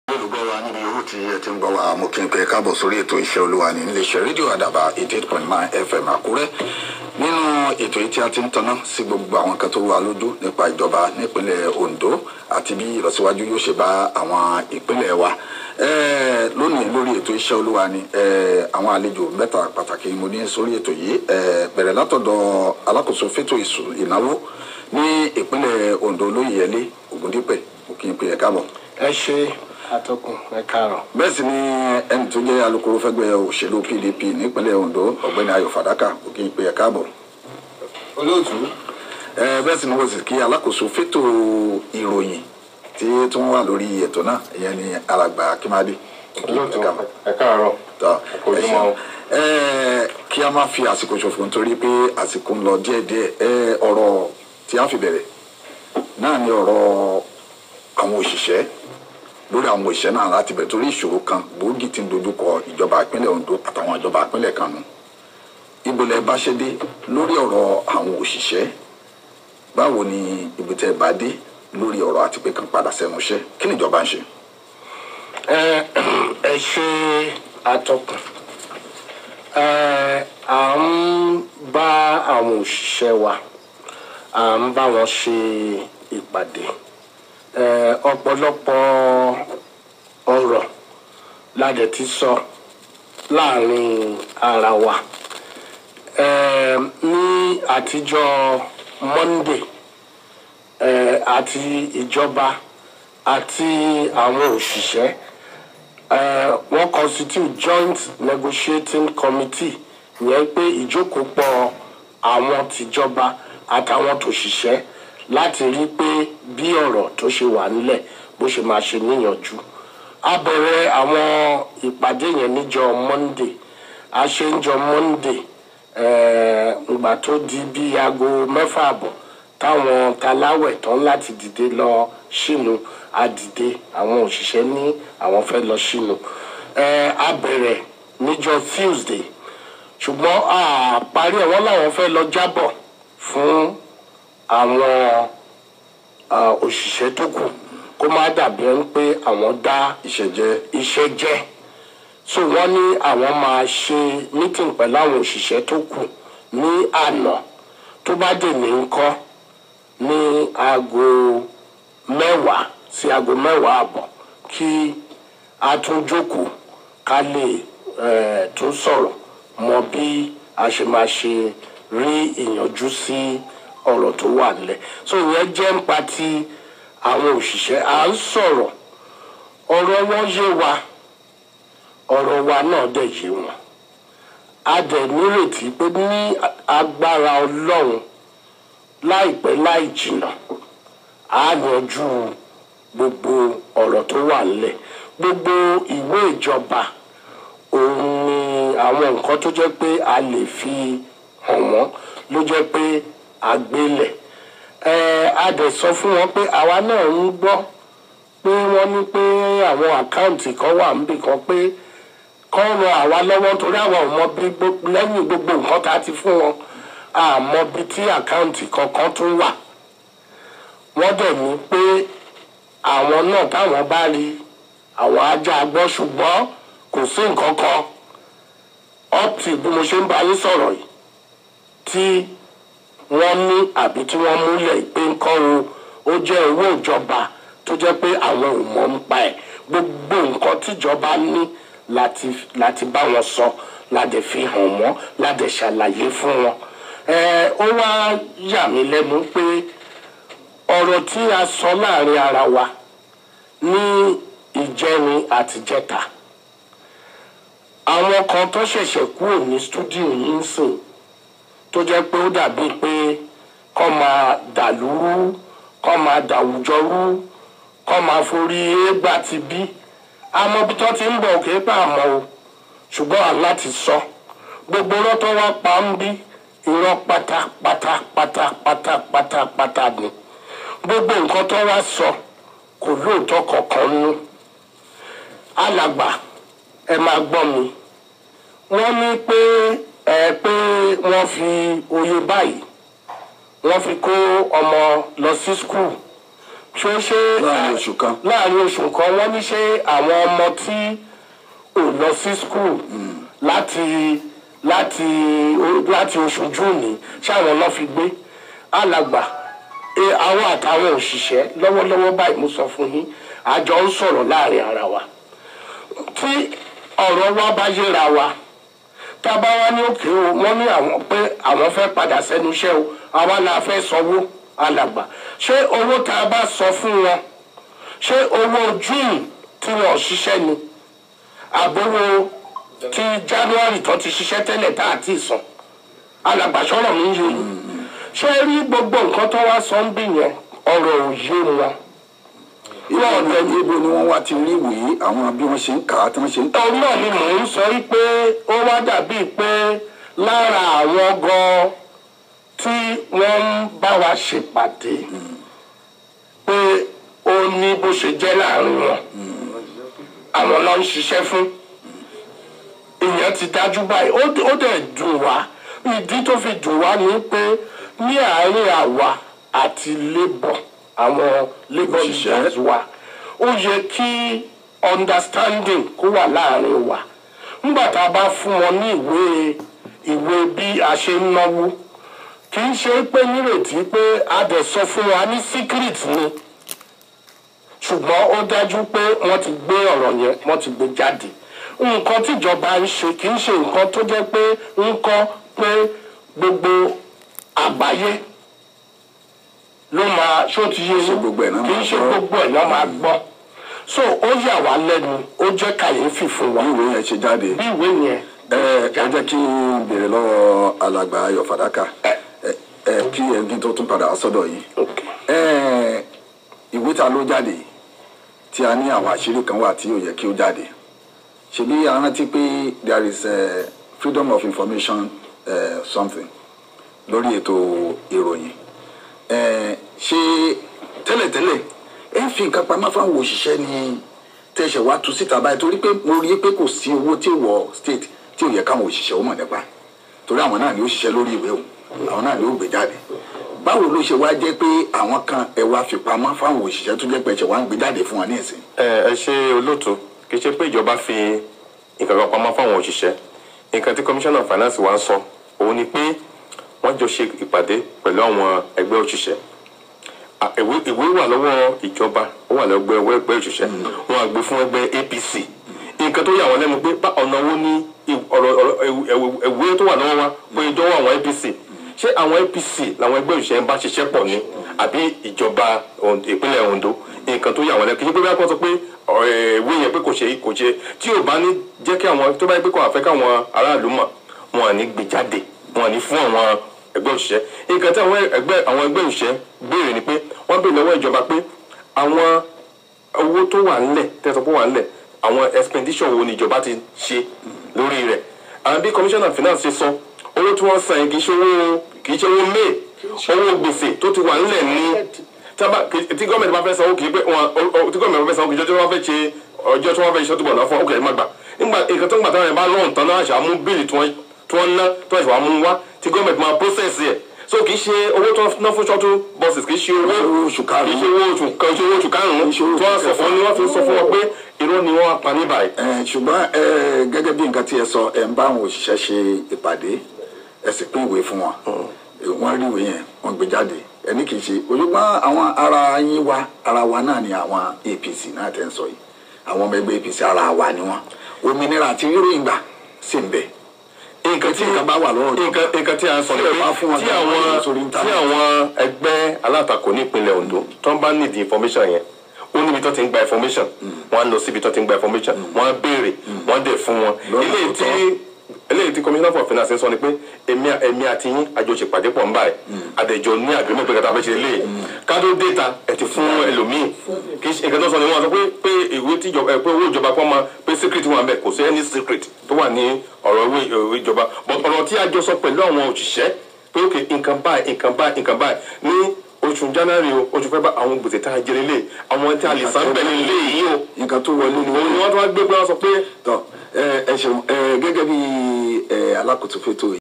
ADABA FM 94.5 Akure, interview on ODSG spending
Introduction to the radio programme, featuring Chief Yele Ogundipe, Commissioner of Finance; Kayode Akinmade, Commissioner of Information & Ayo Fadaka, Director of Media & Publicity in PDP Ondo State.